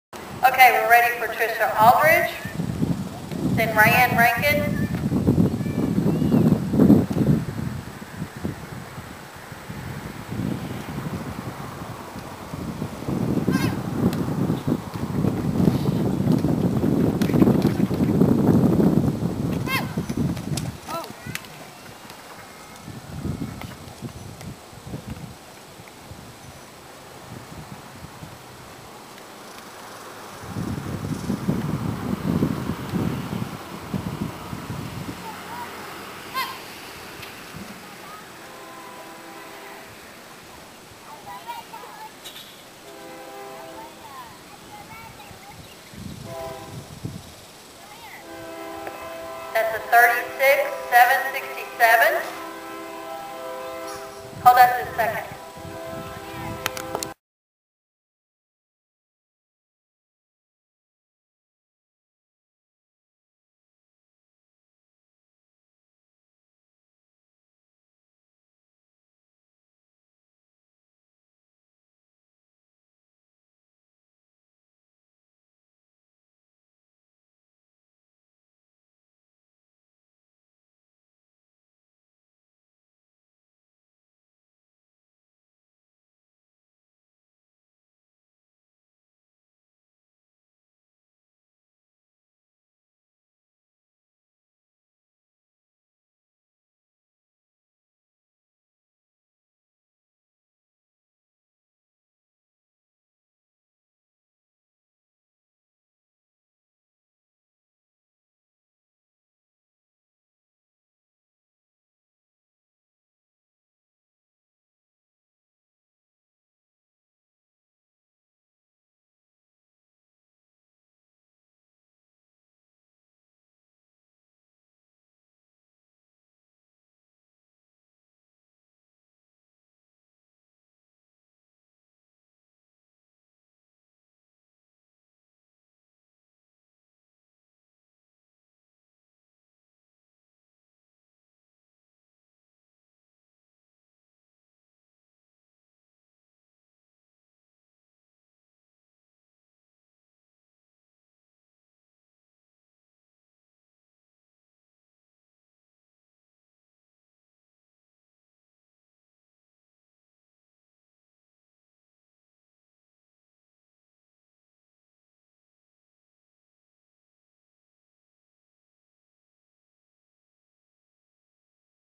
Tricia Aldridge T-R Star Ranch Horse Barrel Racing Training Barrel Horse steps on front foot
Watch until the end, shown in slow motion and frame by frame.  Tricia Aldridge training a horse to run barrels, horse steps on front hoof going around second barrel.  Tricia manages to stay balanced and pulls him out of it.